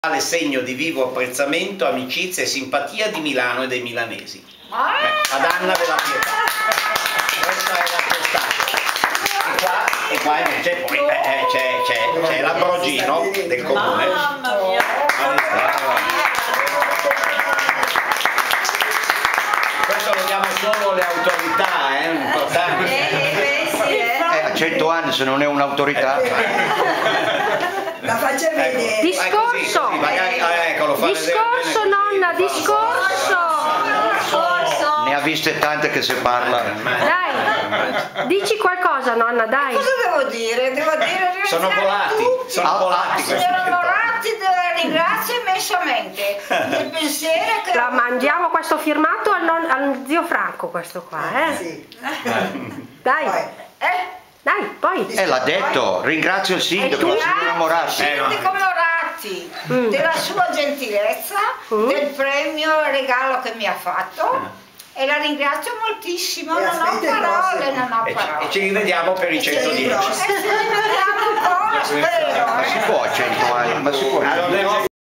Cento candeline con l'Ambrogino
Milano, 21 ottobre 2010 - La signora Anna  Della Pietà ha ricevuto l'Ambrogino d'oro per i suoi primi 100 anni da Pietro Viola, Presidente di  Zona 3